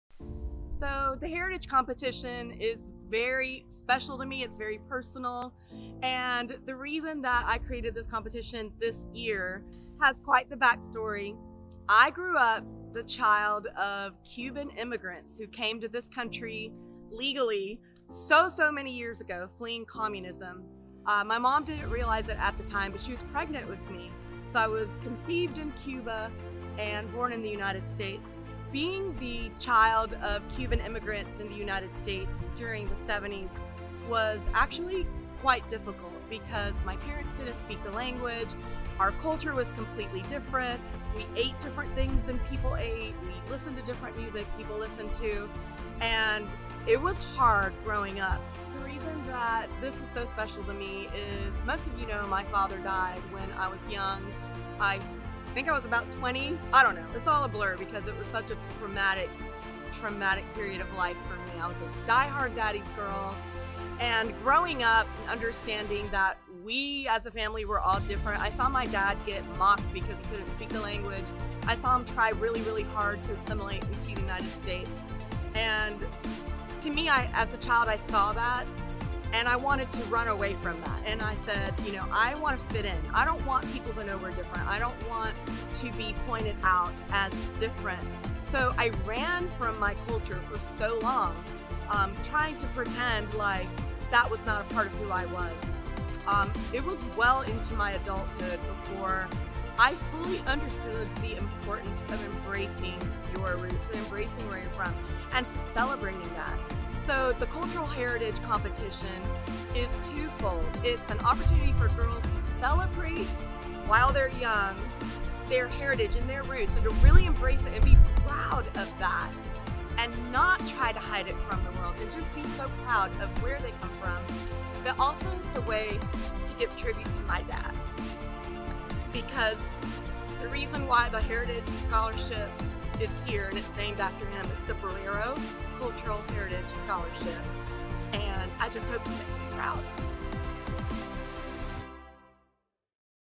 New Heritage Competition unveiled at UNM Nationals
Our national director explains why she created the Cultural Heritage optional competition, and what it personally means to her.